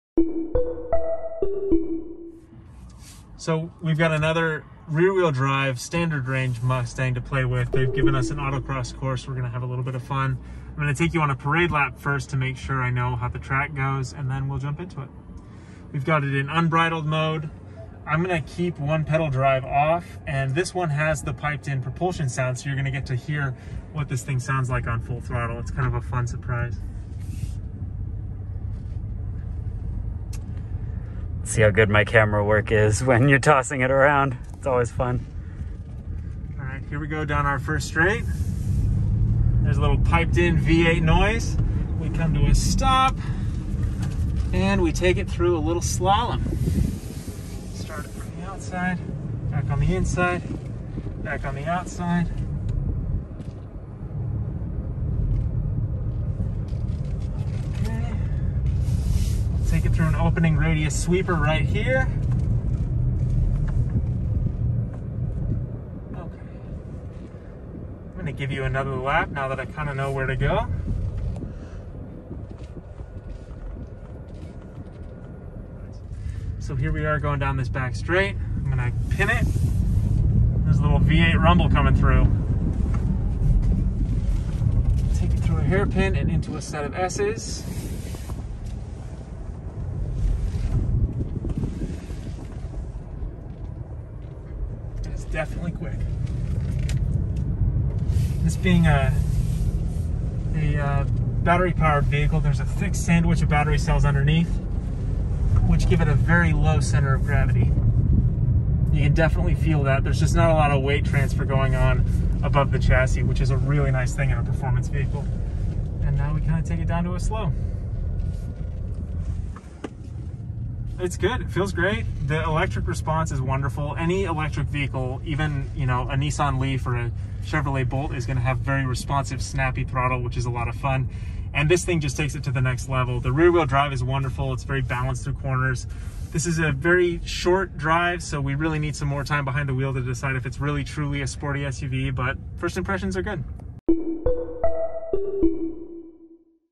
2021 Ford Mustang Mach-E Autocross Hot Laps
We take the 2021 Ford Mustang Mach-E around a quick autocross course to find out how it handles itself in a performance driving scenario.